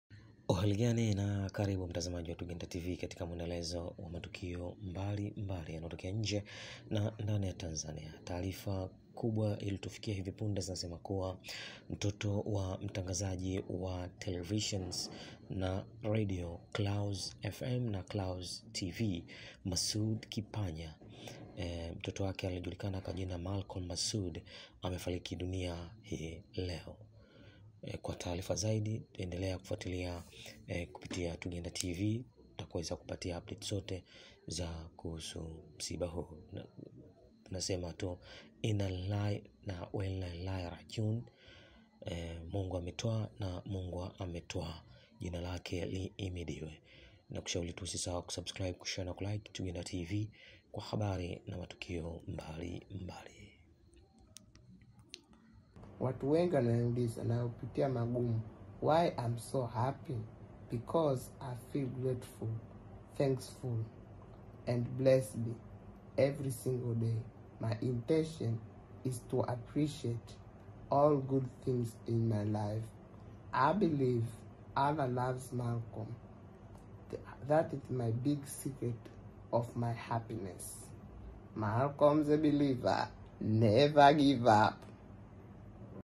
BREAKINGNEWS MTOTO WA MTANGAZAJI MASOUD KIPANYA AFARIKI DUNIA
Mtoto wa Mtangazaji wa television na redio, Masoud Kipanya Malcom Masoud amefariki dunia leo.